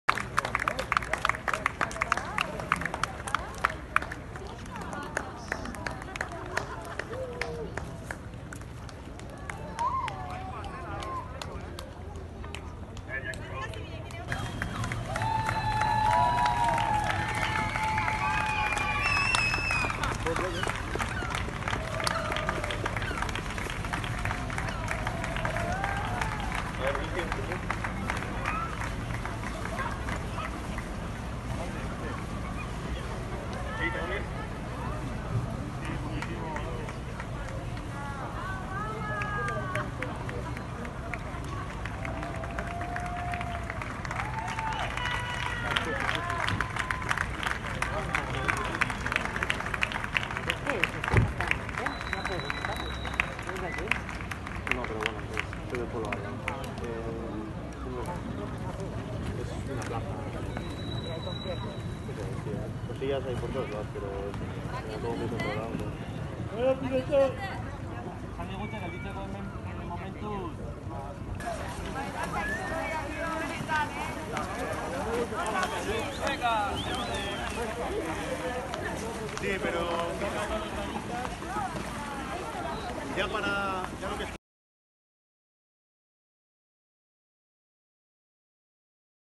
Piraten abordatze sinbolikoa
| Aurten ez da Aste Nagusirik izan Donostian; piratak ere egoerara egokitu dira. Sinbolikoki bada ere, Donostia abordatu dute
beste behin. Pirata Setioa izenez antolatu dute aurtengoan jai egitaraua, ekintza guztiak egun bakar baterako prestatuta.